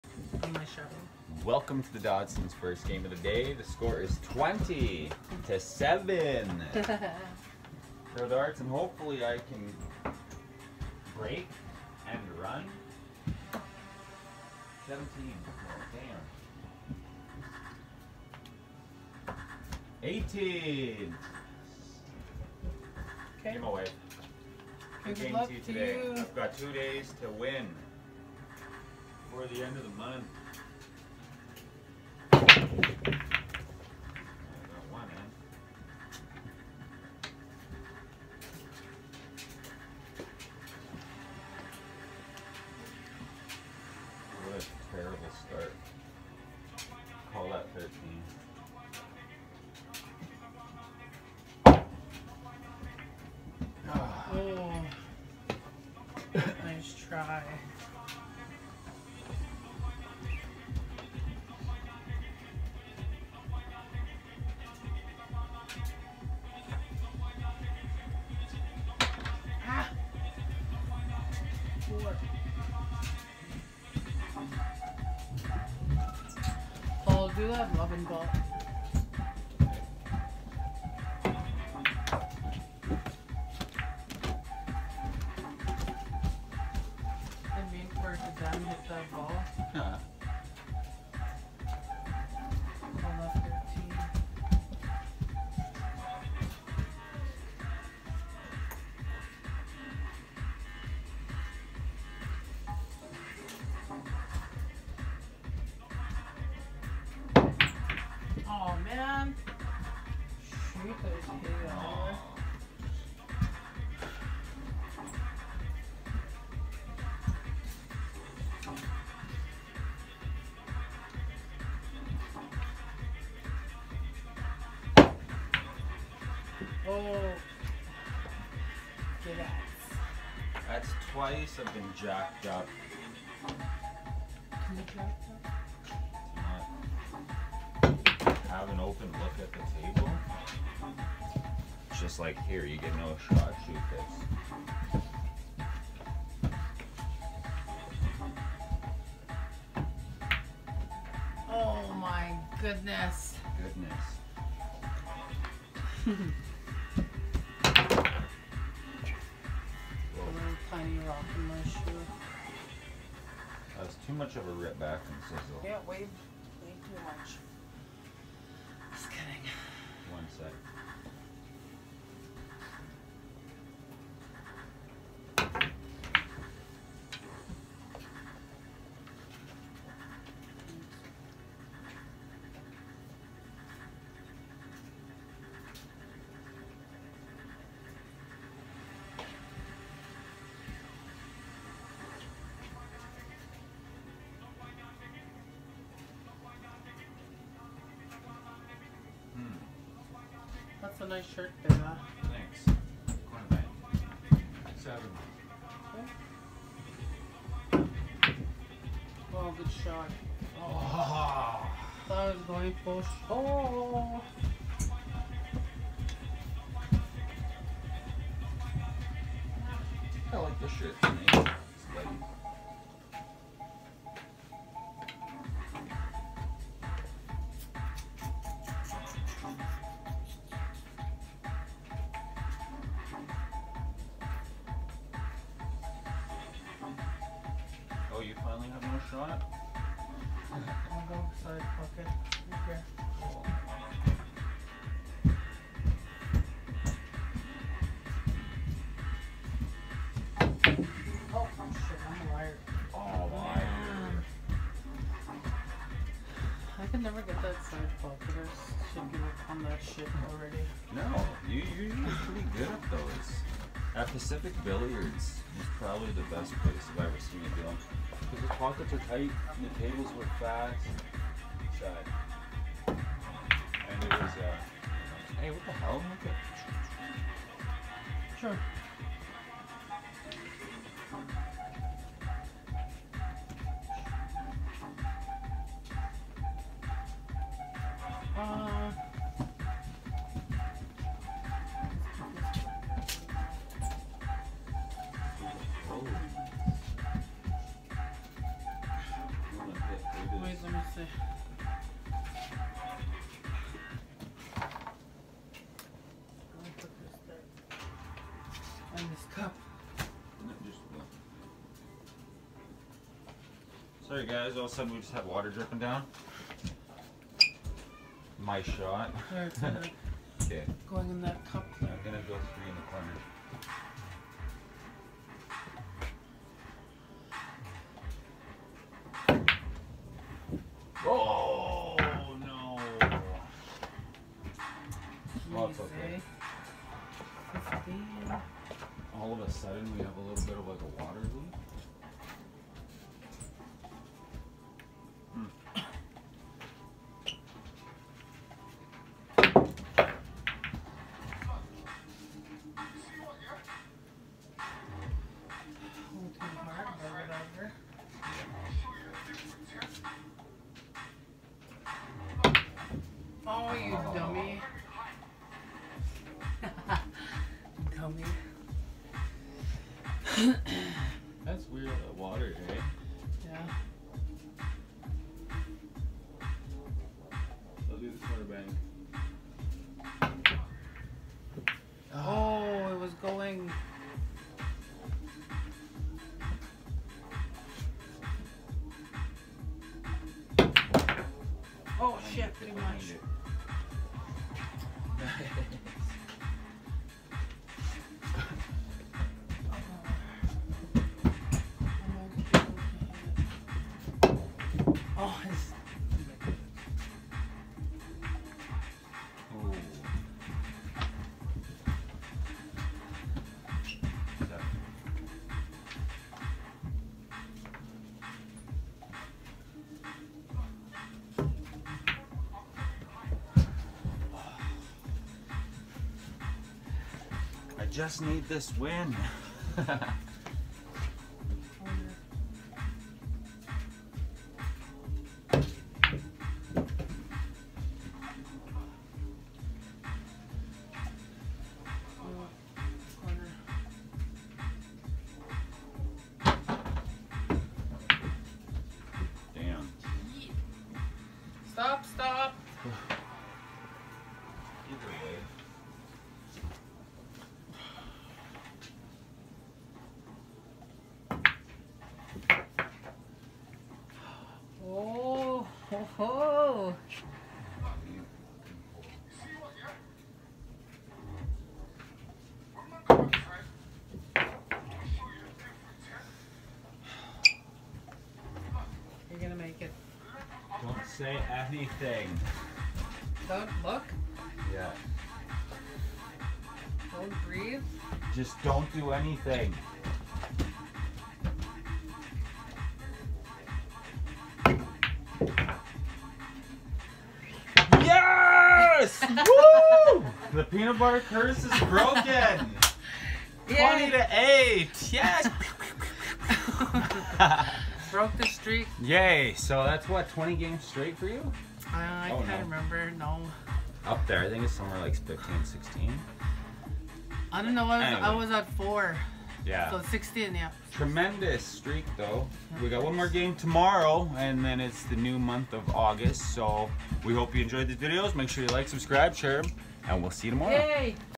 The Dodsons first game of the day. S4 E28
Still haven’t been able to break the “Peanut Butter” curse. The score is 20-7 for my wife.